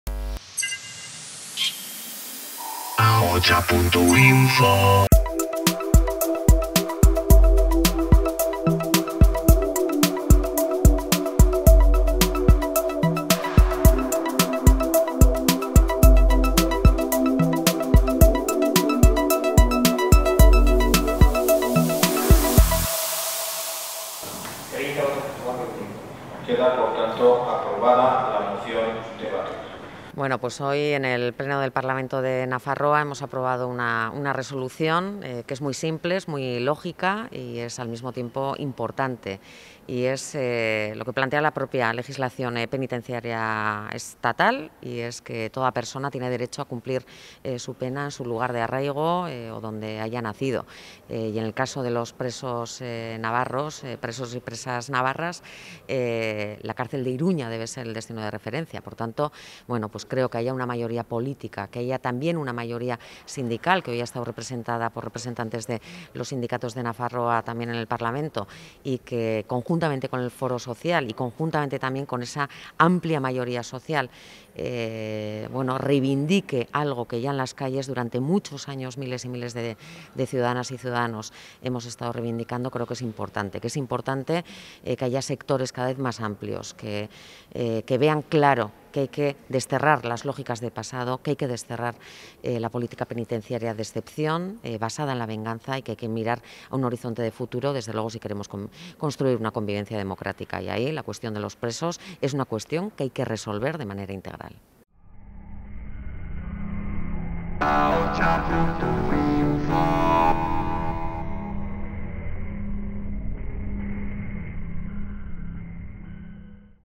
El Parlamento Foral aprueba una moción a favor del acercamiento de presos
Todos los grupos parlamentarios han votado a favor de dicha moción a excepción de Navarra Suma. La coalición derechista se ha enrocado en su falso y repetitivo discurso de “defensa de las víctimas” y ha demostrado su cada vez mayor aislamiento y desconexión de la vida real. La moción ha sido apoyada por el Foro Social y por todos los sindicatos representativos en Nafarroa, cuyos representantes han acudido al pleno.